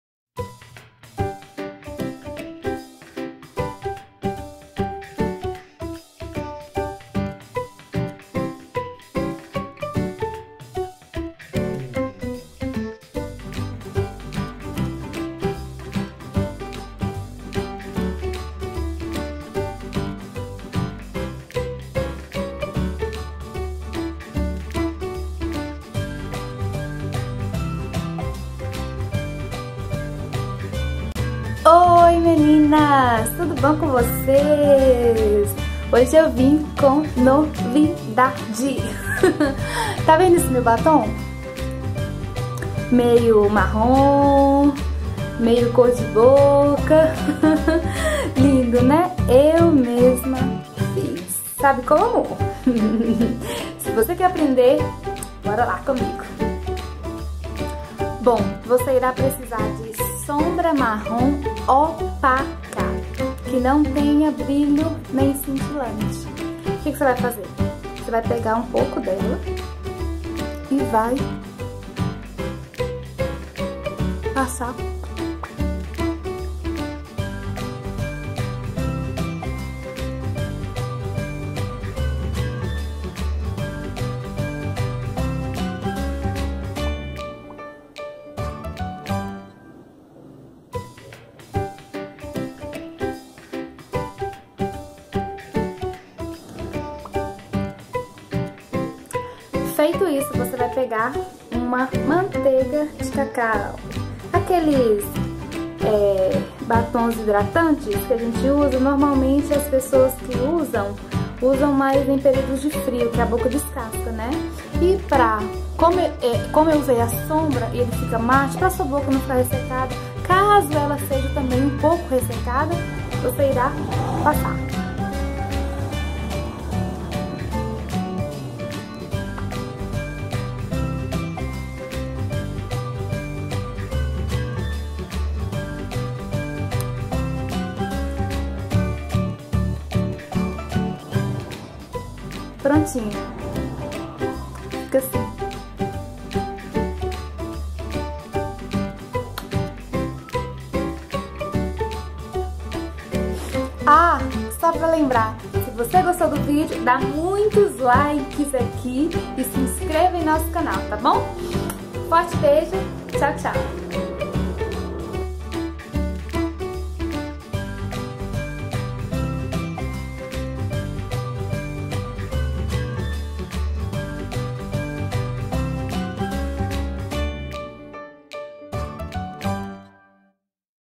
Faça você mesma : batom matte 💄💄💄
Oiii meninas, sabe quando você está arrumando você quer usar um batom mas não tem aquela cor? Isso aconteceu comigo, por isso tive essa ideia que amei fazer é espero que vocês gostem, não deixem de comentar, e dar um curti pra mim! Beijinhos 👌👏😍😘